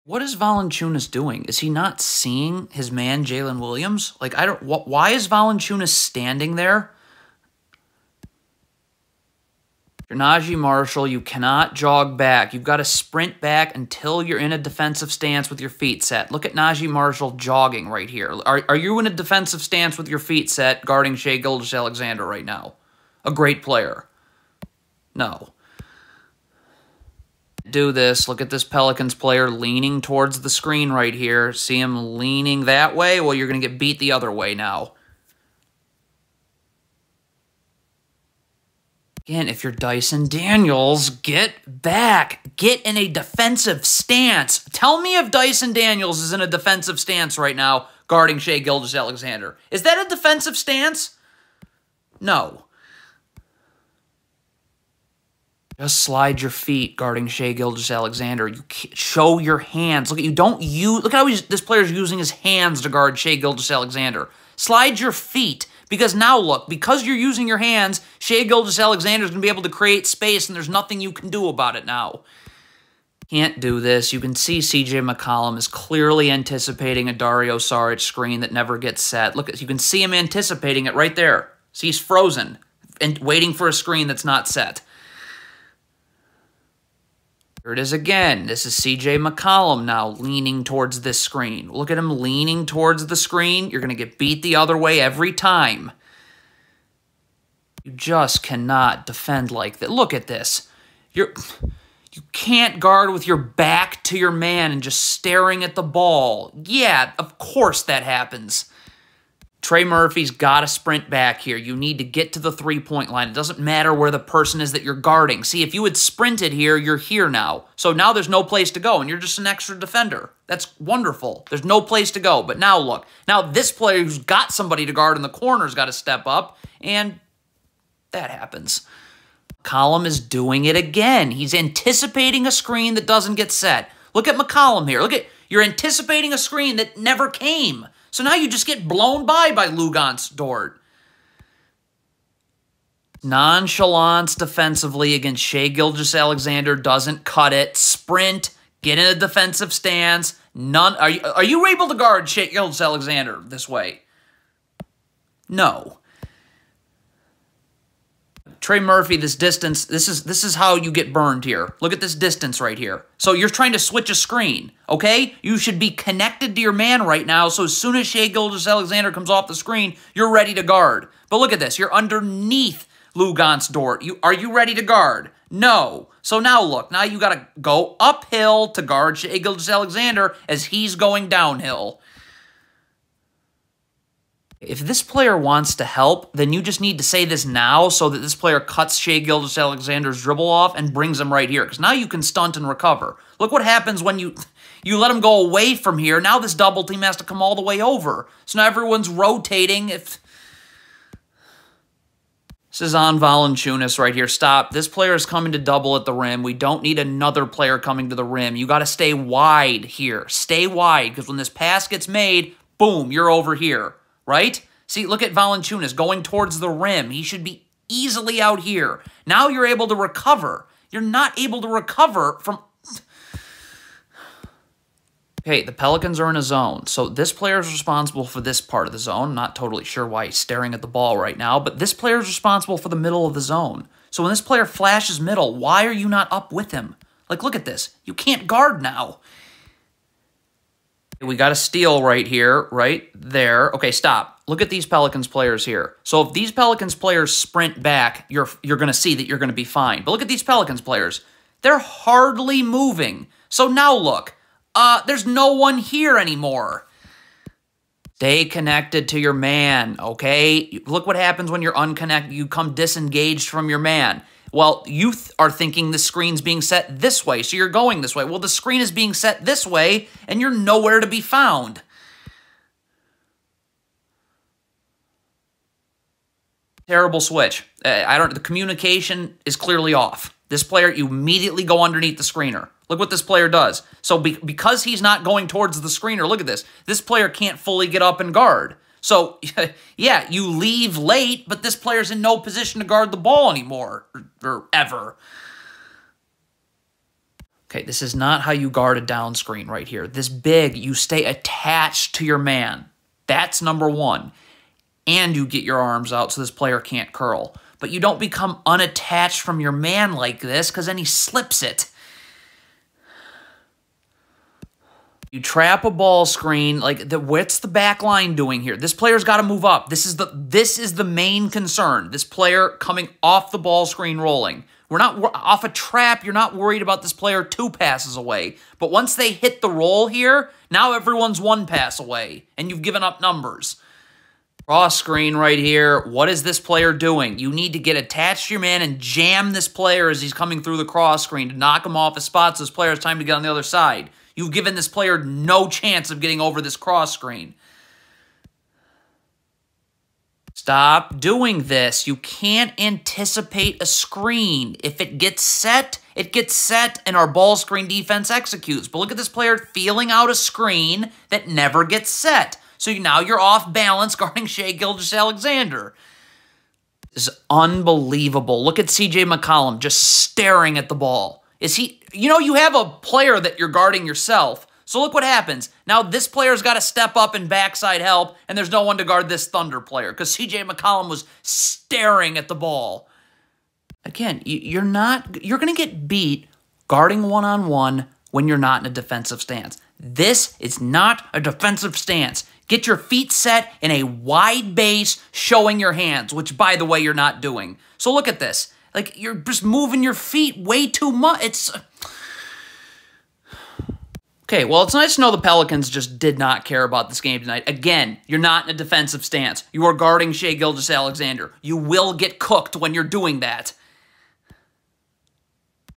PELICANS DEFENSIVE MISTAKES vs. THUNDER | March 11, 2023
Oklahoma City Thunder 110 - New Orleans Pelicans 96